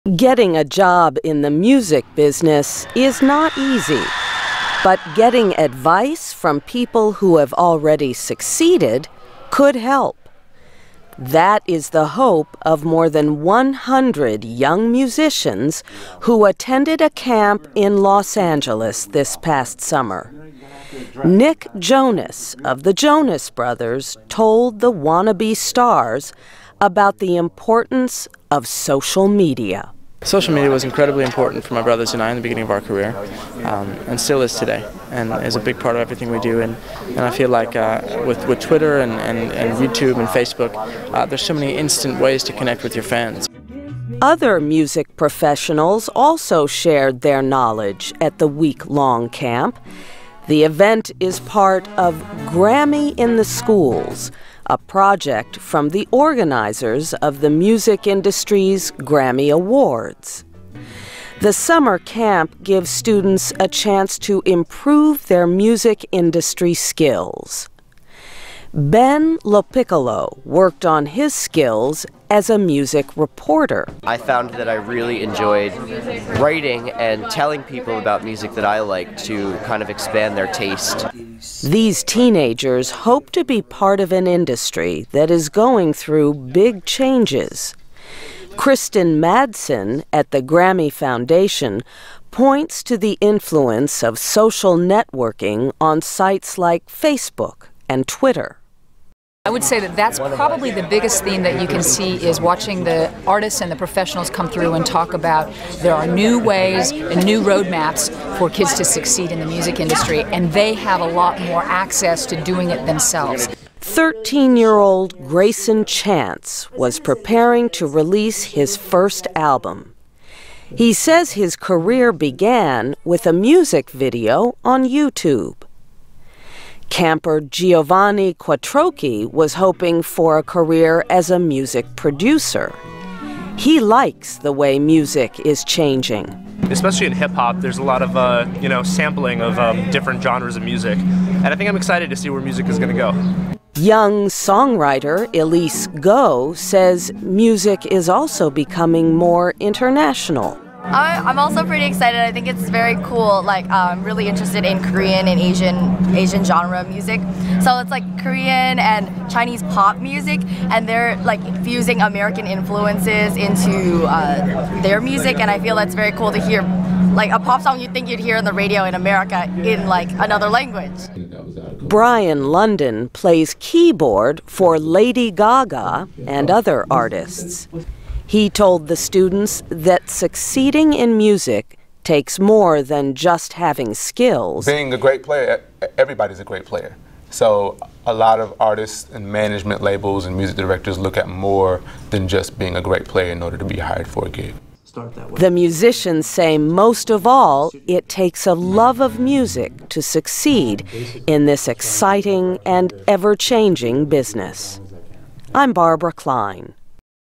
A Camp for Future Grammy Winners
Getting a job in the music business is not easy. But getting advice from people who have already succeeded could help. That is the hope of more than one hundred young musicians who attended a camp in Los Angeles this past summer. Nick Jonas of the Jonas Brothers told the wannabe stars about the importance of social media.

NICK JONAS: "Social media was incredibly important for my brothers and I at the beginning of our career and still is today. With Twitter and YouTube and Facebook, there are so many instant ways to connect with your fans."

Other music professionals also shared their knowledge at the week-long camp.  The event is part of "Grammy in the Schools," a project from the organizers of the music industry's Grammy Awards.

The summer camp gives students a chance to improve their music industry skills. Ben LoPiccolo worked on his skills as a music reporter.

BEN LOPICCOLO: "I found that I really enjoyed writing and telling people about music that I like to kind of expand their taste."

These teenagers hope to be part of an industry that is going through big changes. Kristen Madsen at the Grammy Foundation points to the influence of social networking on sites like Facebook and Twitter.

KRISTEN MADSEN: "I would say that that's probably the biggest theme that you can see, is watching the artists and the professionals come through and talk about, there are new ways and new roadmaps for kids to succeed in the music industry, and they have a lot more access to doing it themselves."

Thirteen-year-old Greyson Chance was preparing to release his first album. He says his career began with a music video on YouTube.

Camper Giovanni Quattrochi was hoping for a career as a music producer. He likes the way music is changing.

GIOVANNI QUATTROCHI: "Especially with hip-hop, there's a lot of sampling of different genres of music.  And I think I'm excited to see where music is going to go."

Young songwriter Elise Go says music is also becoming more international.

LISE GO: ""I'm also pretty excited. I think it's very cool, like I'm really interested in Korean and Asian-genre music. So it's like Korean and Chinese pop music, and they're like fusing American influences into their music, and I feel that's very cool to hear, like a pop song you think you'd hear on the radio in America, in like another language."

Brian London plays keyboard for Lady Gaga and other artists. He told the students that succeeding in music takes more than just having skills.

BRIAN LONDON: "Being a great player -- everybody's a great player. So a lot of artists, management labels and music directors look at more than just being a great player in order to be hired for a gig."

The musicians say most of all it takes a love of music to succeed in this exciting and ever changing business. I'm Barbara Klein.